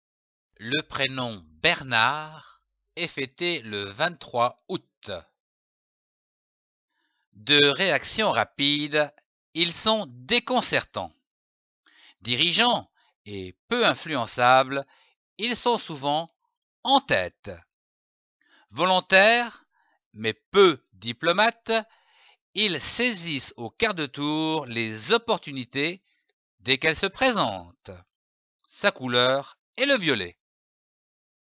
Choisir et fêter un prénom de garçon : Bernard, 23 août
Choisir un prénom de garçon pour votre fils à l’aide des grandes lignes de sa caractérologie ou fêter le prénom : Bernard, 23 août . A partager et envoyer : De réactions rapides, ils sont déconcertants. Dirigeants et peu influençable, ils sont souvent en tête. Volontaires mais peu diplomates, ils saisissent au quart de tour les opportunités dès qu’elles se présentent. Sa couleur est le violet.